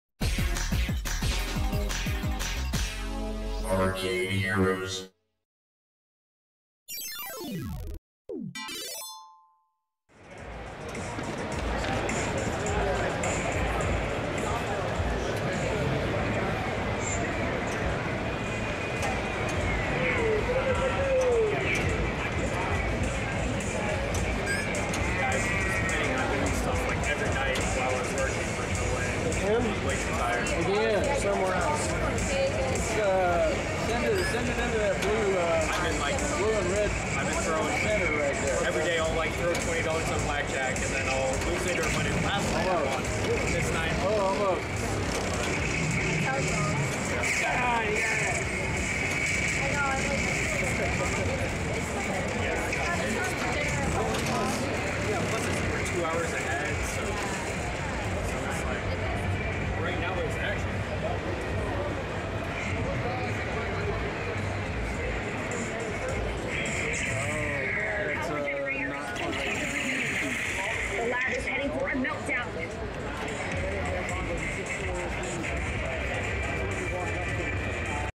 A Brief Visit To The American Pinball Booth, Amusement Expo 2023
- Another quickie here, taking a look at American Pinball's booth which featured Galactic Tank Force, Legends Of Valhalla and Hot Wheels. FOR LONGER VIDEOS OF THE GAMES THEMSELVES, click on the Card links or search my channel. This video is more just to give a quick idea of the booth, not to focus on the games.

Help Arcade Heroes out by purchasing a product with one of our designs on it